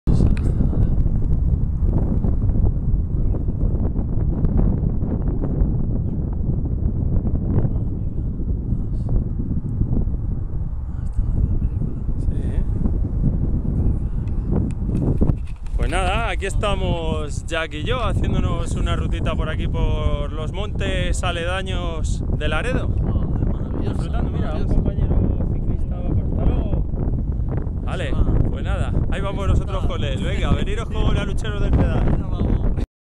Btt por los alrededores de Laredo
Haciendo btt por los montes que rodean Laredo. Comenzaremos por el trazado de la ruta Carlos V, subiremos a Seña disfrutando de unas maravillosas vistas de todo Laredo y bajaremos a Liendo llegando hasta la playa de San Julian. Por los acantilados volveremos a Laredo y su puerto pesquero.
Información de la ruta, track GPS, perfiles, fotos y descripción